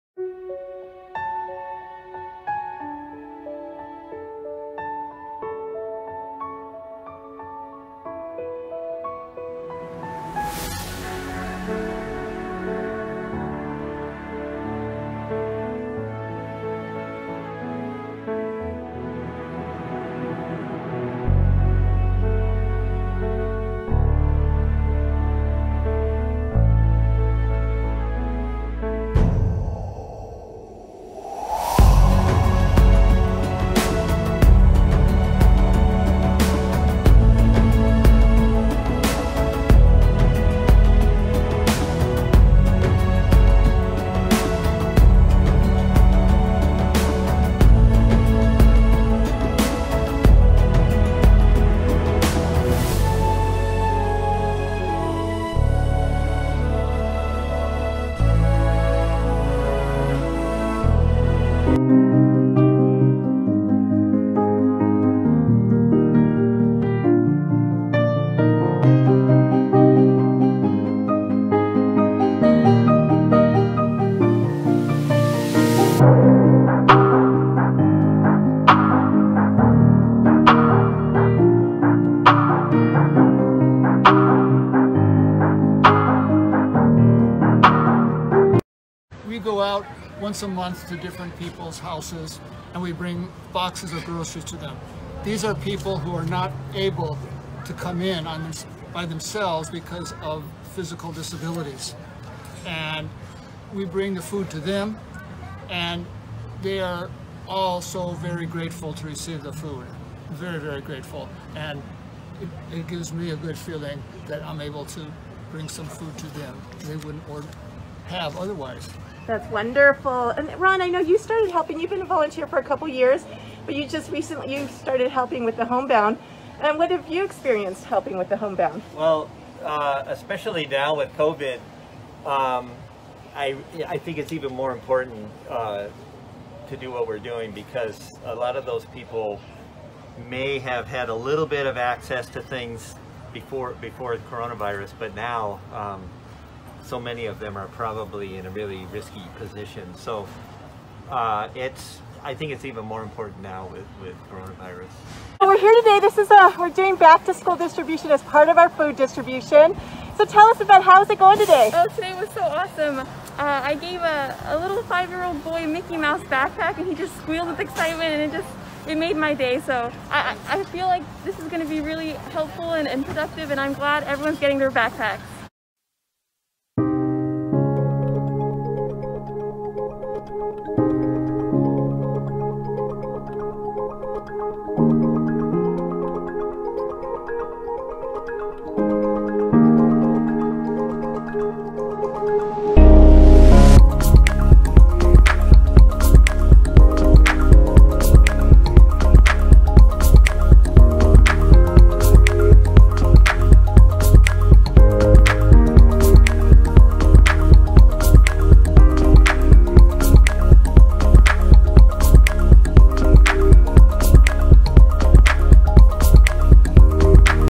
2020 Highlights!
Thank you for your continued support! We were able to modify and continue many of our core and special programs, like our Back to School, Thanksgiving, and Holiday Distributions during 2020. We are grateful to continue helping every neighbor in need. Help us prepare for 2021! Thank You!